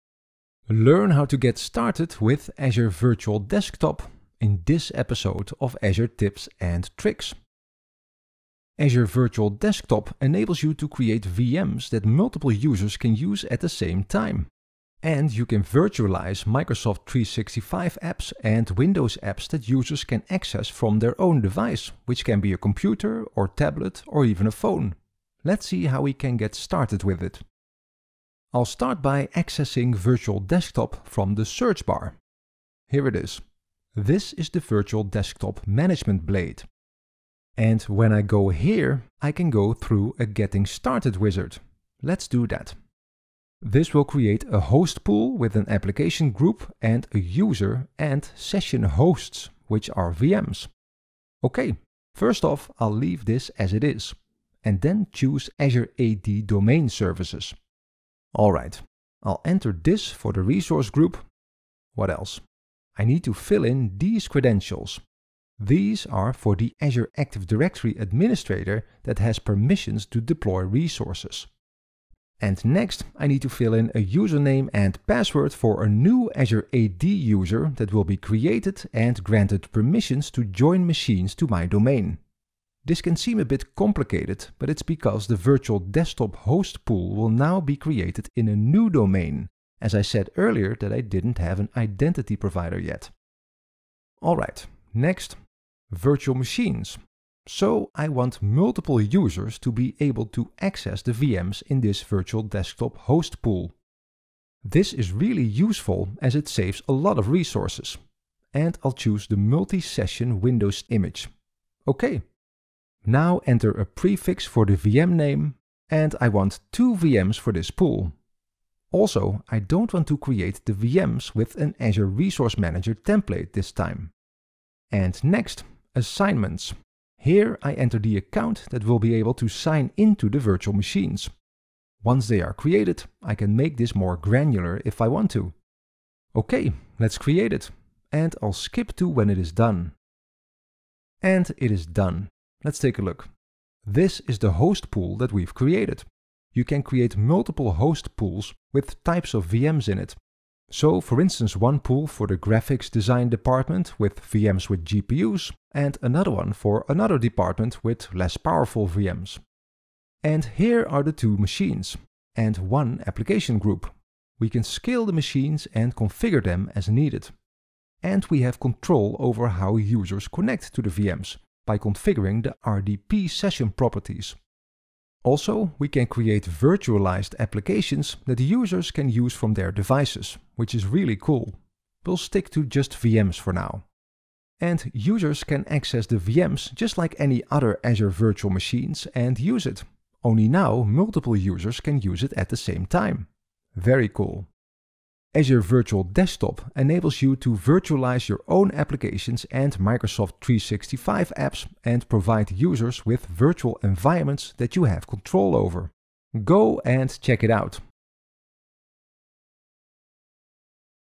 How to get started with Azure Virtual Desktop | Azure Tips and Tricks
In this edition of Azure Tips and Tricks, learn how to get started with Azure Virtual Desktop.

Get started with 12 months of free services and $200 USD in credit.